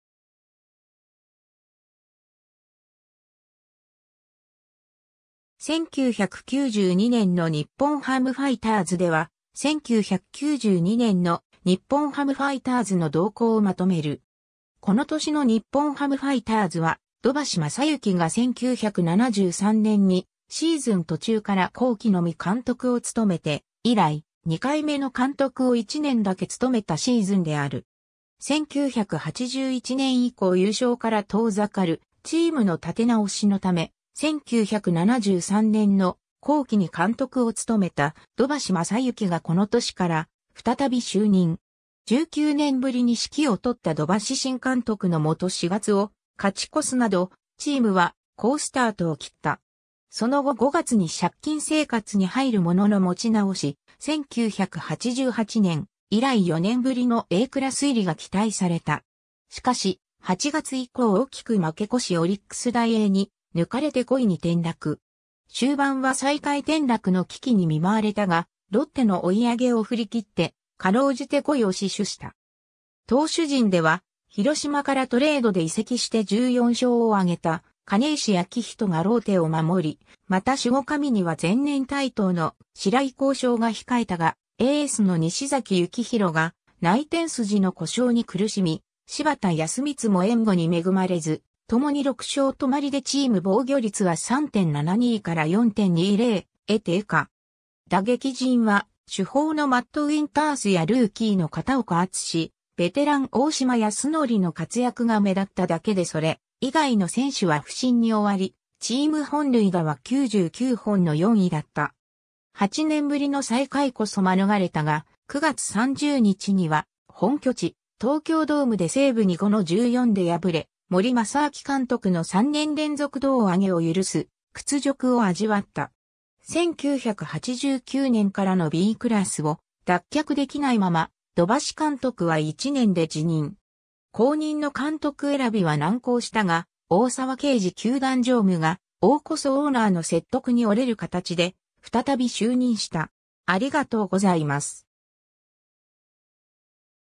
1992年の日本ハムファイターズ
1992年の日本ハムファイターズ（1992ねんのにほんハムファイターズ）では、1992年の日本ハムファイターズの動向をまとめる。
 この年の日本ハムファイターズは、土橋正幸が1973年にシーズン途中から後期のみ監督を務めて以来、2回目の監督を1年だけ務めたシーズンである。
 1981年以降優勝から遠ざかるチームの立て直しのため、1973年の後期に監督を務めた土橋正幸がこの年から再び就任。
19年ぶりに指揮をとった土橋新監督のもと4月を勝ち越すなど、チームは好スタートを切った。
その後5月に借金生活に入るものの持ち直し、1988年以来4年ぶりのAクラス入りが期待された。
しかし、8月以降大きく負け越しオリックス・ダイエーに抜かれて5位に転落。
終盤は最下位転落の危機に見舞われたが、ロッテの追い上げを振り切って辛うじて5位を死守した。
投手陣では広島からトレードで移籍して14勝をあげた金石昭人がローテを守り、また守護神には前年台頭の白井康勝が控えたが、エースの西崎幸広が内転筋の故障に苦しみ、柴田保光も援護に恵まれず、共に6勝止まりでチーム防御率は3.72から4.20へ低下。
打撃陣は主砲のマット・ウインタースやルーキーの片岡篤史、ベテラン大島康徳の活躍が目立っただけでそれ以外の選手は不振に終わり、チーム本塁打は99本の4位だった。
8年ぶりの最下位こそ免れたが、9月30日には本拠地・東京ドームで西武に5-14で敗れ、森祇晶監督の3年連続胴上げを許す屈辱を味わった。
1989年からのBクラスを脱却できないまま、土橋監督は1年で辞任。
後任の監督選びは難航したが、大沢啓二球団常務が大社オーナーの説得に折れる形で再び就任した。